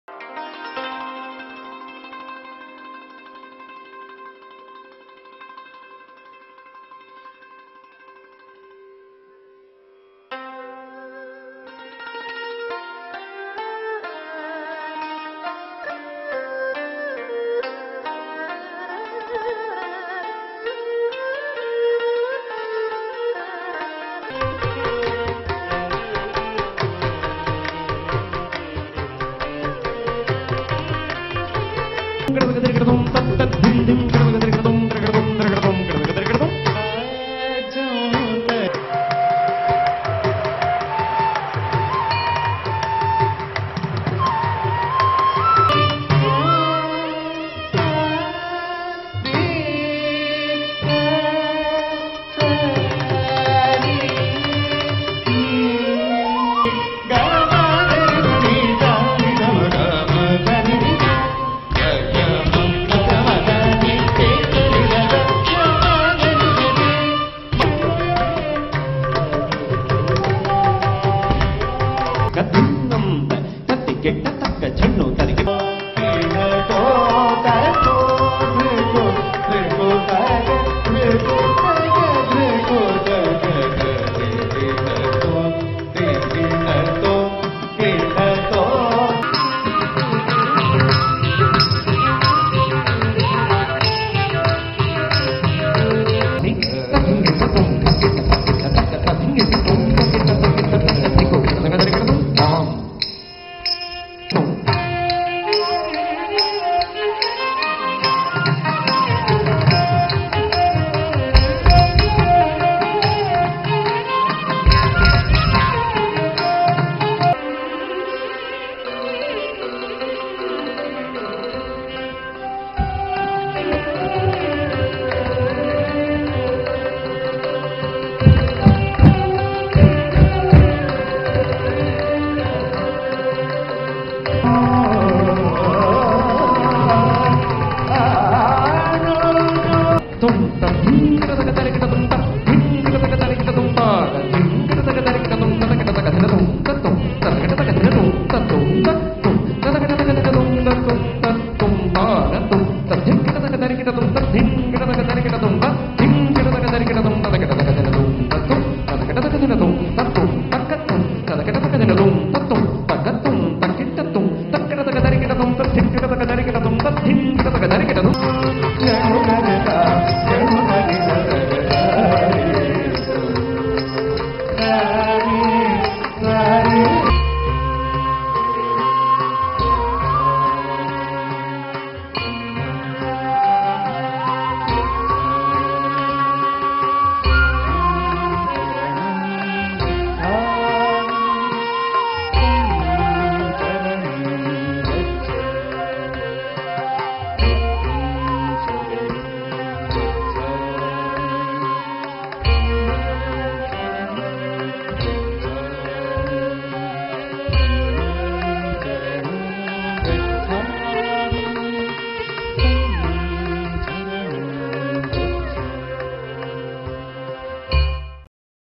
Anjasa Offical Trailor Video
"Anajsa" - Unravel the wonders of Buddhist monuments of Asia by Apsaras Arts Singapore

Artistic Mentorship & Direction - Neila Sathyalingam

Concept,Music, Choreography Design - Aravinth Kumarasamy
Creative Direction

Choreographer & Leading Artiste - Mohanapriyan Thavarajah